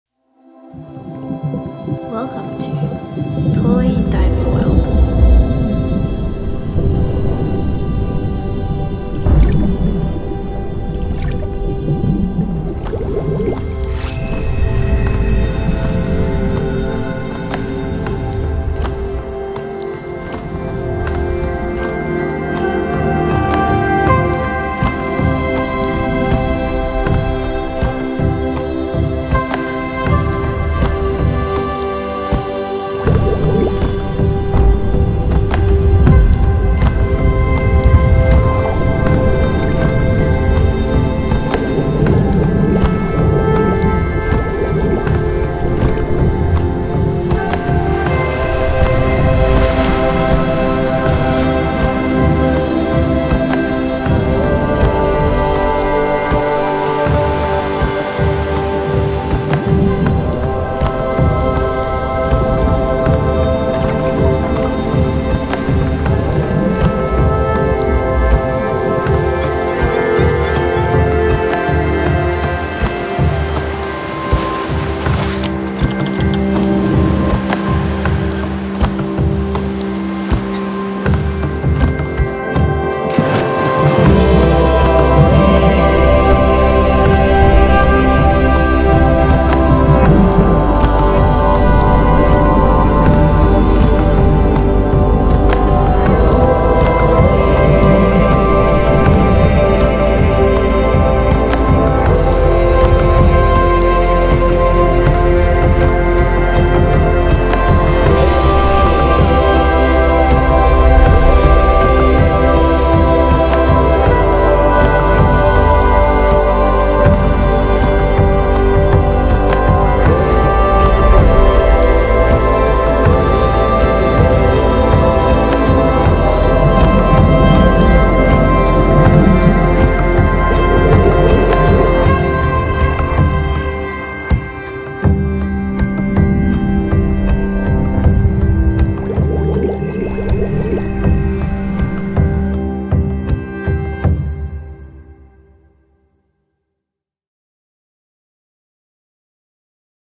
Toi diving water world | 静岡県伊豆市土肥の水中世界 | Sony α7S III
静岡県伊豆市土肥のダイビングでの水中世界を撮影しました！

第2作品目
- Toi diving water world -
【静岡県伊豆市土肥の美しい水中世界】

Director / Camera / Edit : Reiya Sato
Drone pilot : Reiya Sato